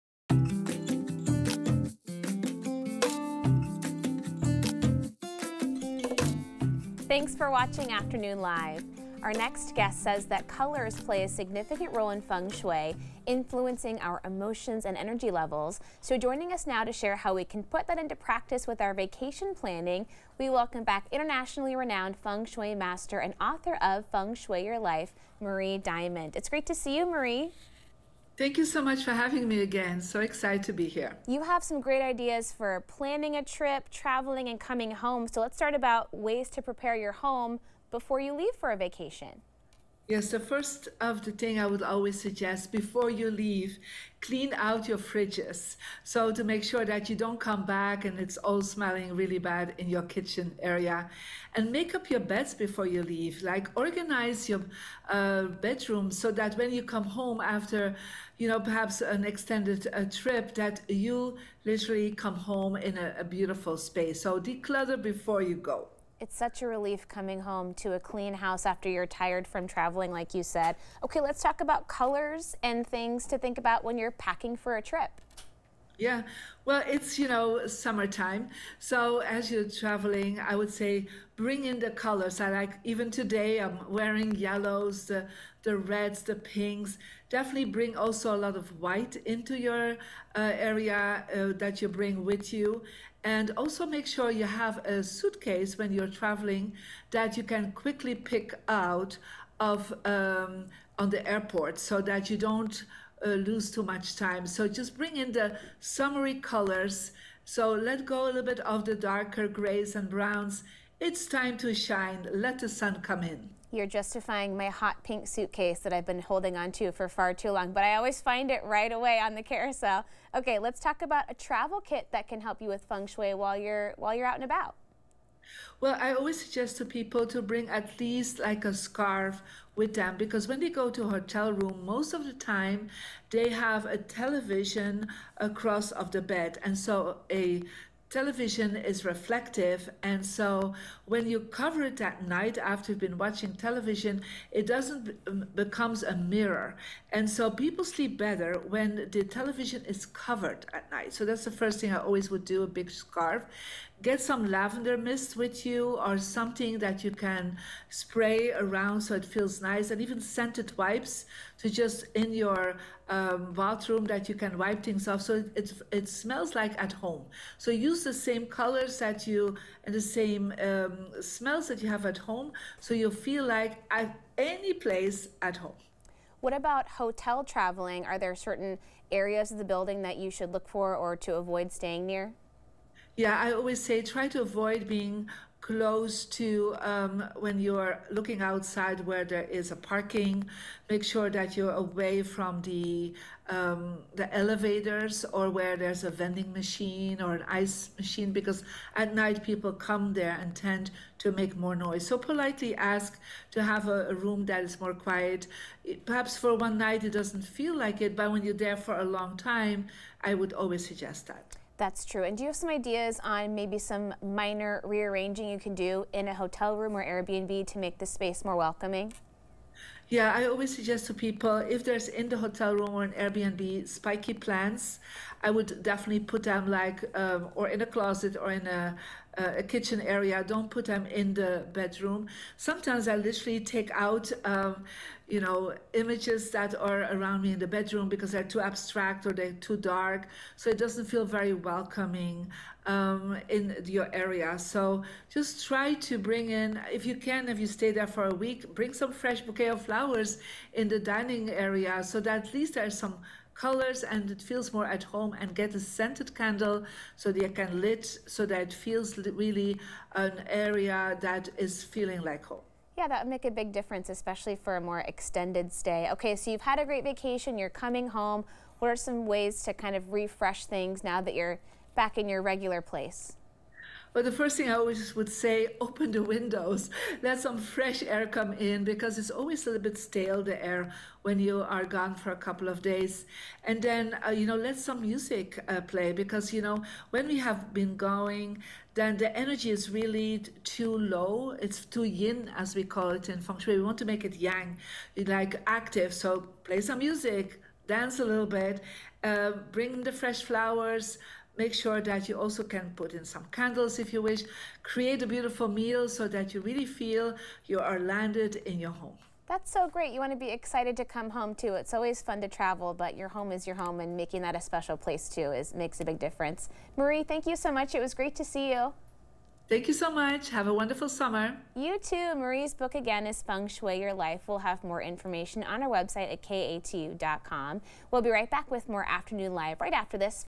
Marie Diamond Shares Tips to Feng Shui Your Vacation
Marie Diamond, world-renowned Feng Shui master and author of "Feng Shui Your Life," shared how we can put Feng Shui into practice with our vacation planning to help influence our emotions and energy levels.